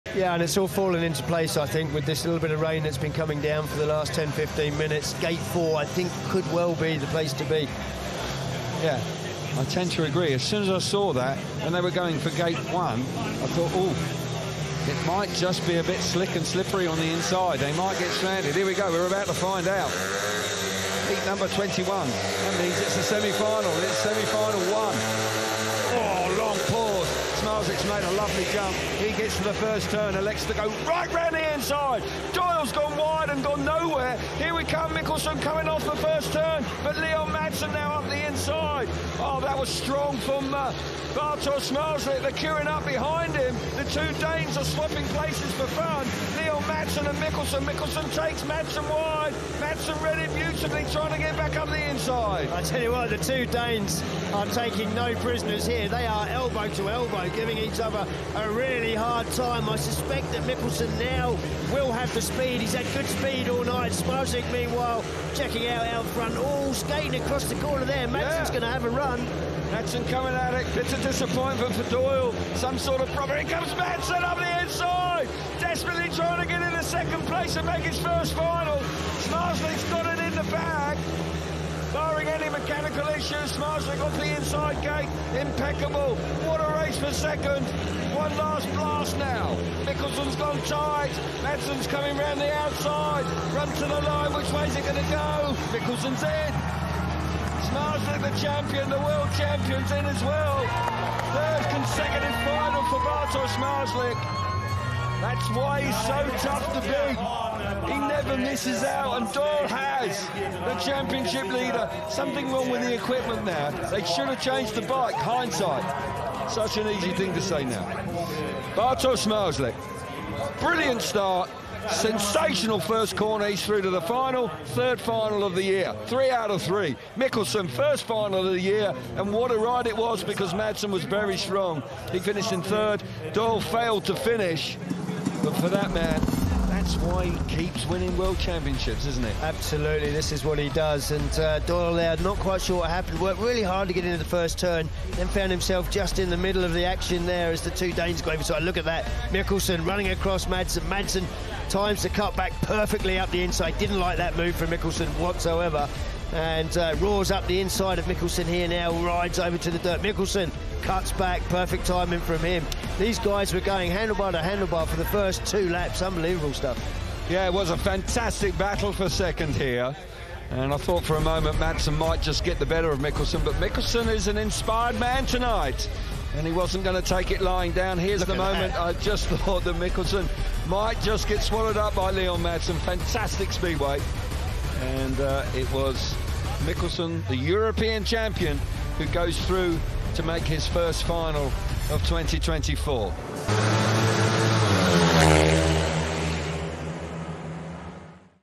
The Danish Duel 🤺 Semi-Final 1 GermanSGP 2024 | FIM Speedway Grand Prix
No national love here 💥

Leon Madsen and Mikkel Michelsen went wheel-to-wheel for the last spot in Final ⚔️